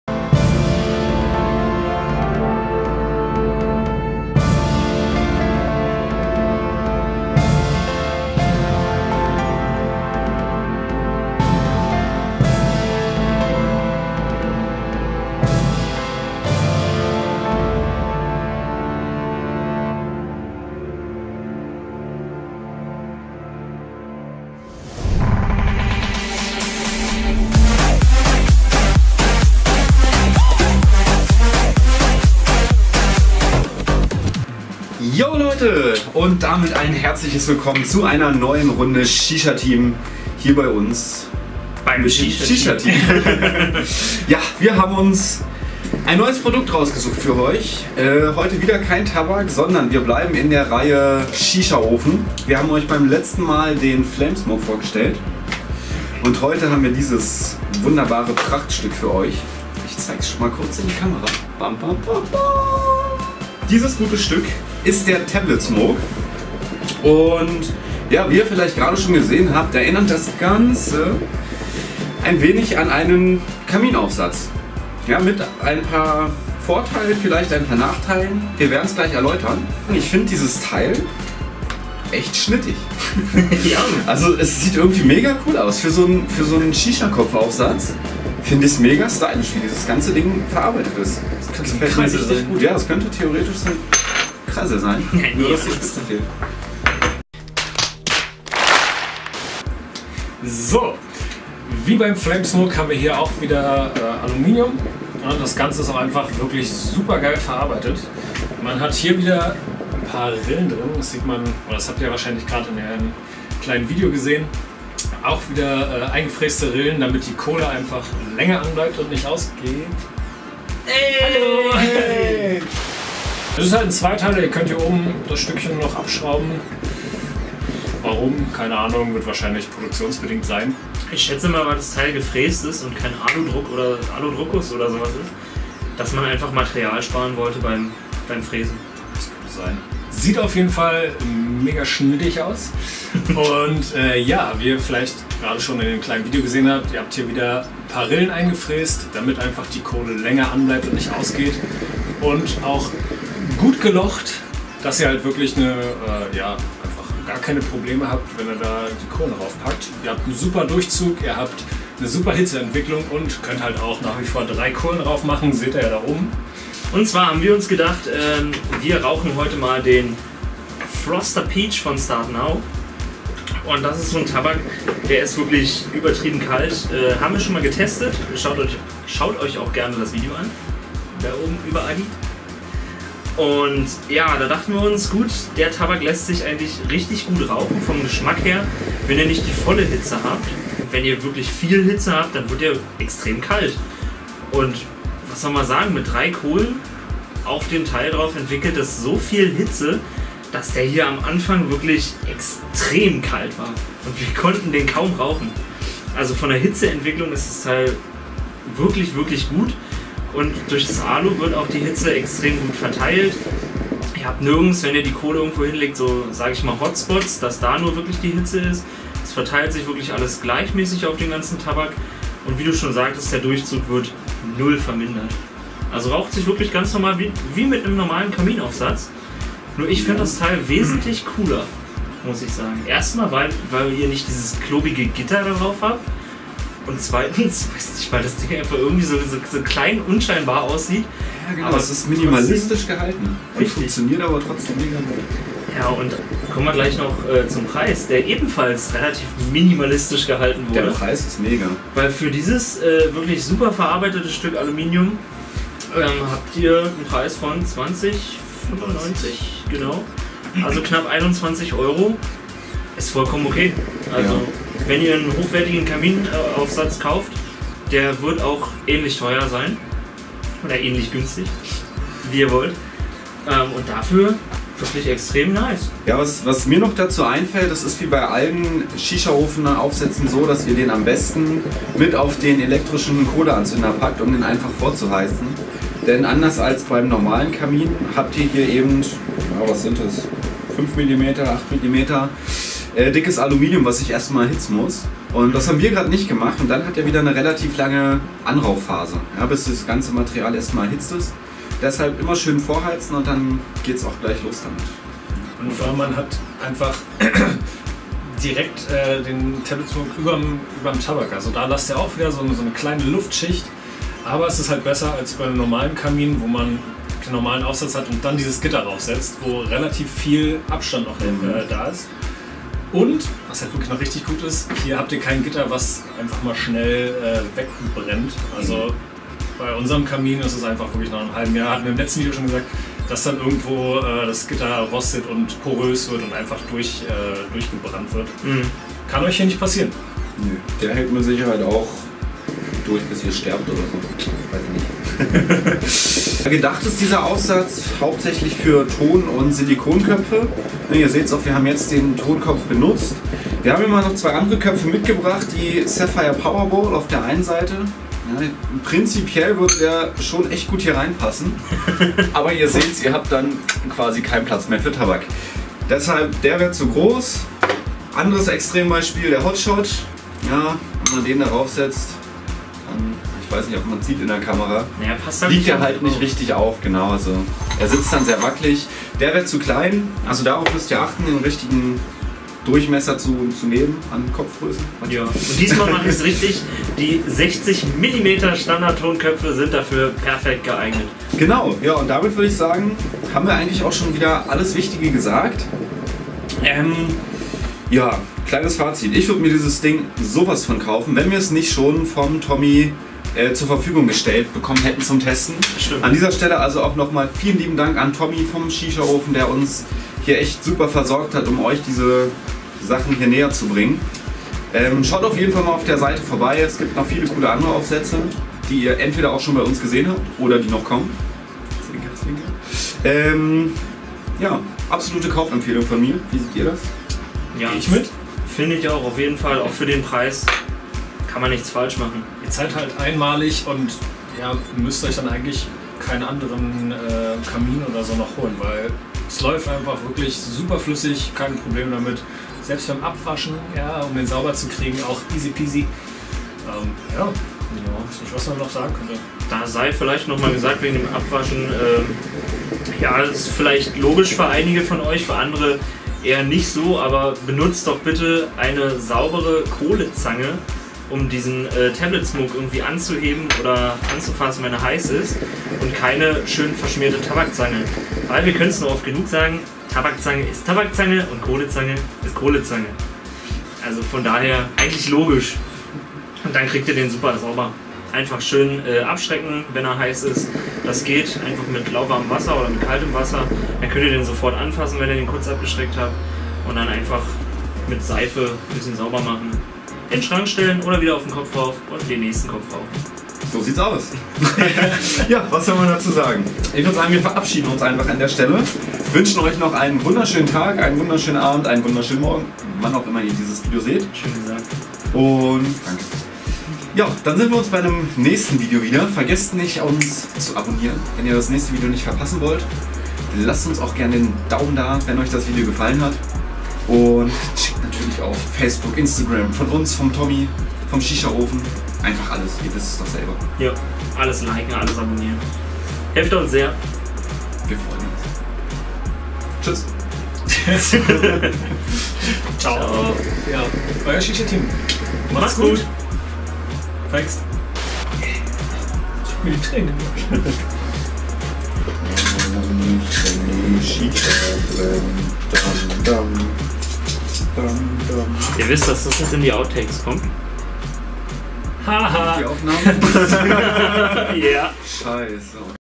Mit dem Tablet Shisha rauchen! powered by ShishaOfen 😎
Hey Leute, huete gibt es den dritten Aufsatz von ShishaOfen, den TablettSmoke. Eine ganz neue Interpretation des altbewährten Kaminaufsatzes! Viel Spaß!

─Setup─
Amy Feather Steel
Tonkopf + TablettSmoke + 3 TOM c26 Kohlen